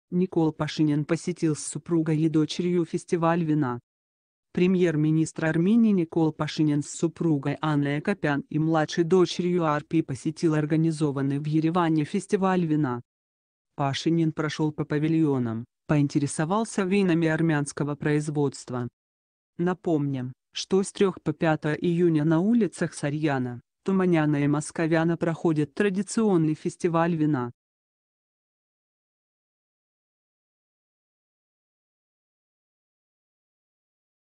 Никол Пашинян посетил с супругой и дочерью фестиваль вина
Премьер-министр Армении Никол Пашинян с супругой Анной Акопян и младшей дочерью Арпи посетил организованный в Ереване фестиваль вина.

Пашинян прошел по павильонам, поинтересовался винами армянского производства.

Напомним, что с 3 по 5 июня на улицах Сарьяна, Туманиана и Московяна проходит традиционный фестиваль вина.

Армения
Арцах
Карабах
Азербайджан
Срочные новости
Экстренные новости
Новости дня
Վերջին լուրեր
Յութուբ լուրեր